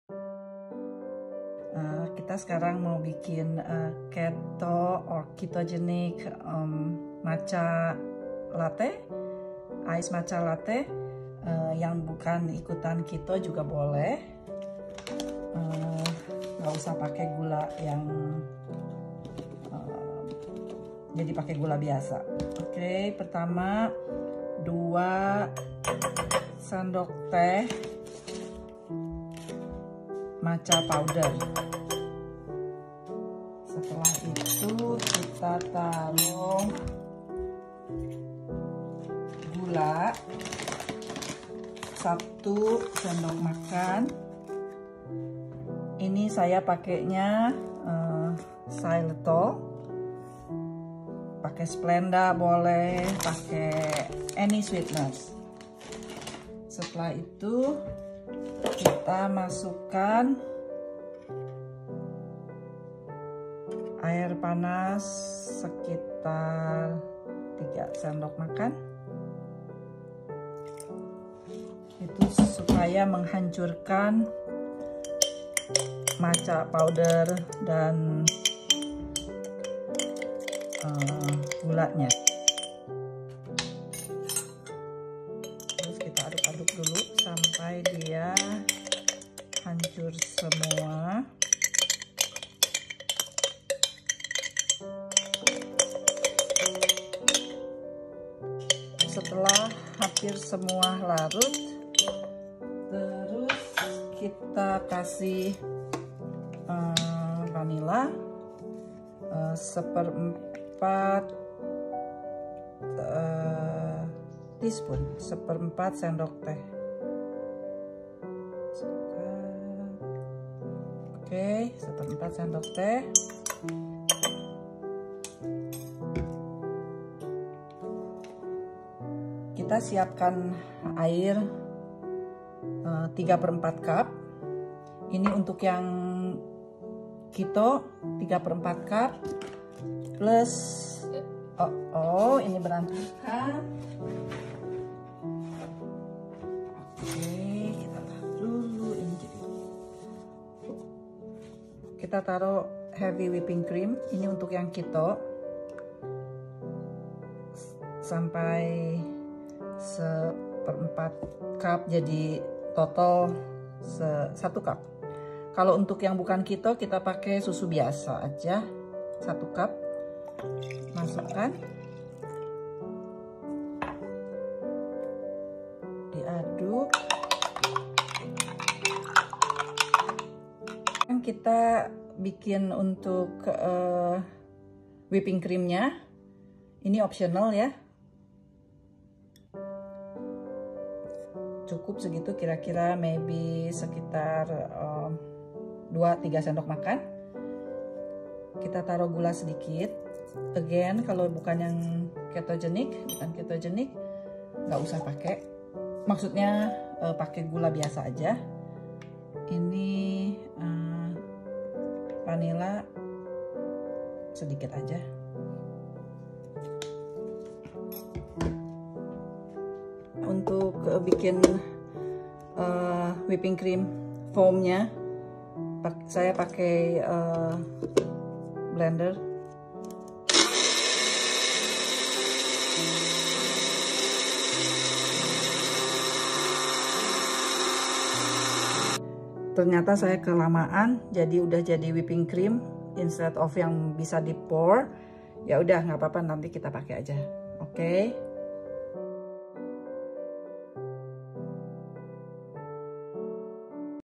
Easy Keto Ice Matcha Latte by Cris' Kitchen non keto option with English subtitle Starbucks copycat
Keto Ice Matcha Latte Recipe by Cris' Kitchen
(turn on your subtitle/closes caption on the top of the video)

Ingredients :
2 tsp matcha powder
1 tbsp sugar substitute ( I used xylitol this time)
3 tbsp hot water
1/4 tsp vanilla
3/4 cup water
1/4 cup heavy whipping cream
Ice cubes

Frothy cream (optional):
2 tbsp heavy whipping cream
1/4 tsp sugar substitute
1/8 tsp vanilla

For non keto or regular option :
Use 1 cup of milk instead of water + heavy whipping cream.
Use regular sugar